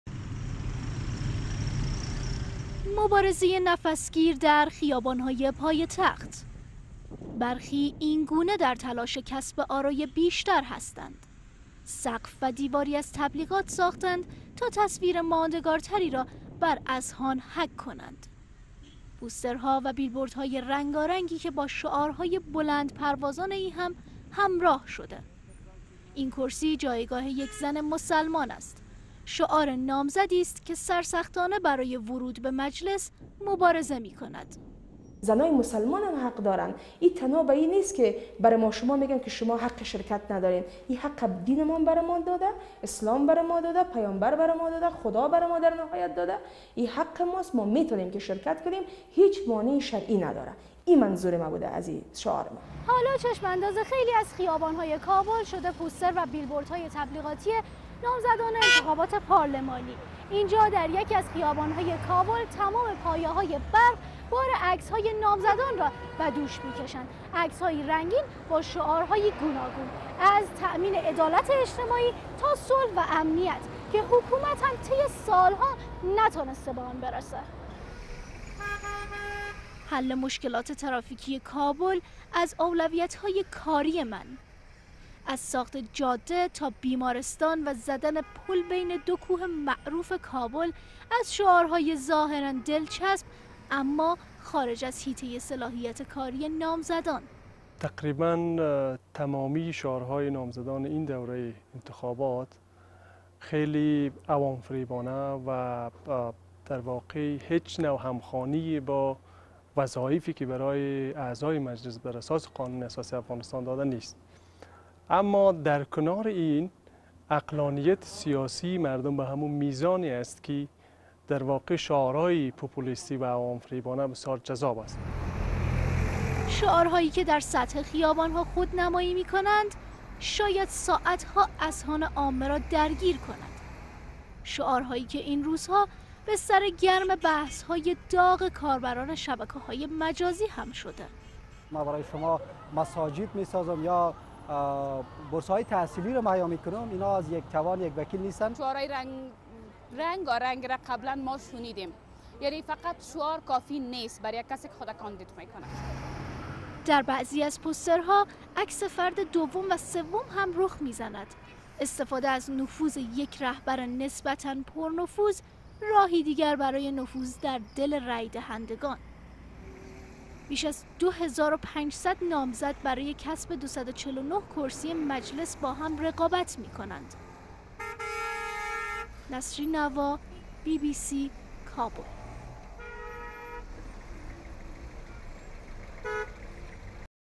بیش از نیم میلیون نفر از فهرست رای دهندگان حذف شدند - انتخابات افغانستان
کمیسیون انتخابات افغانستان بیش از نیم میلیون نفر را از فهرست رای دهندگان حذف کرده. برای انتخابات پارلمانی ۲۸ میزان یا مهر، ۹ و نیم میلیون نفر ثبت نام کرده بودند. بسیاری احزاب سیاسی این رقم را غیر واقعی خوانده بودند.

نسرین نوا، خبرنگار ما در کابل میگوید خیابانهای پایتخت این روزها عرصه رقابت نامزدان برای جلب آرا است.